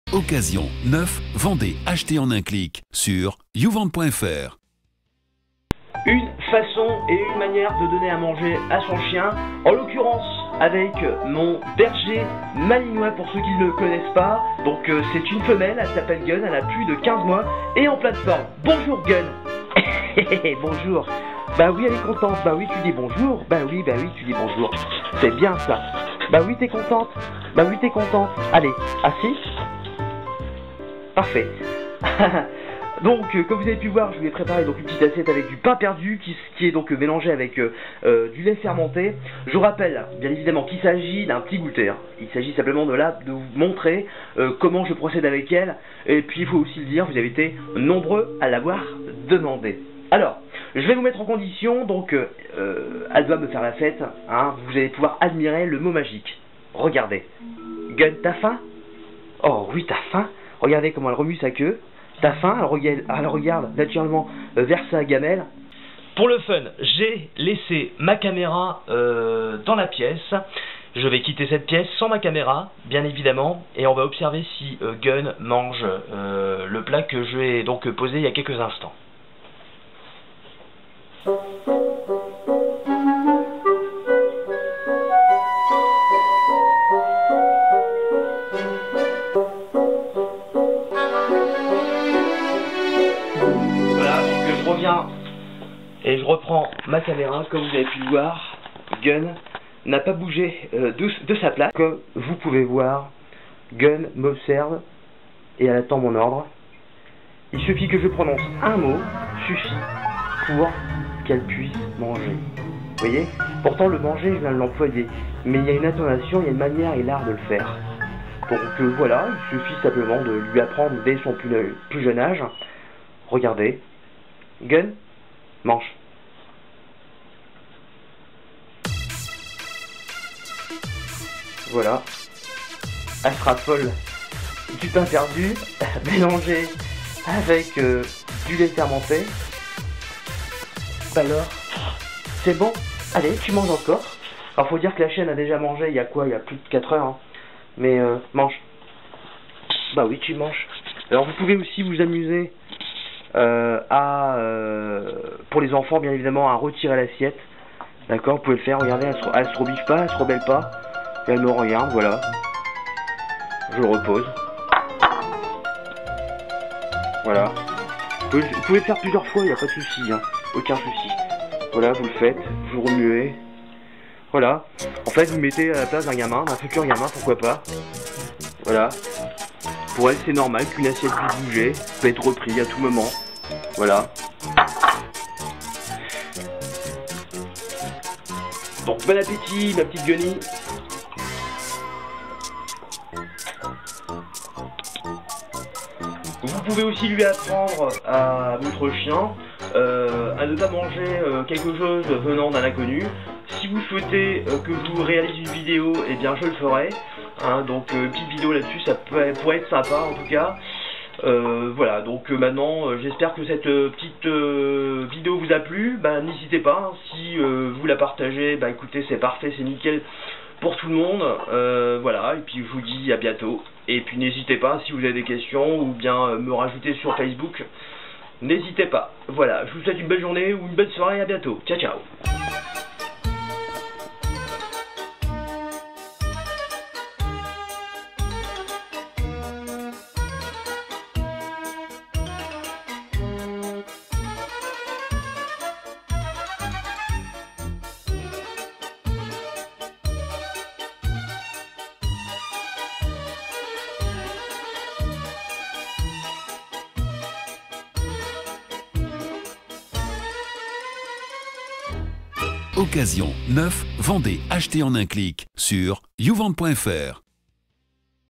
malinois mange quand son maître lui ordonne !
Le berger Malinois mange son plat quand son maître lui donne l'ordre !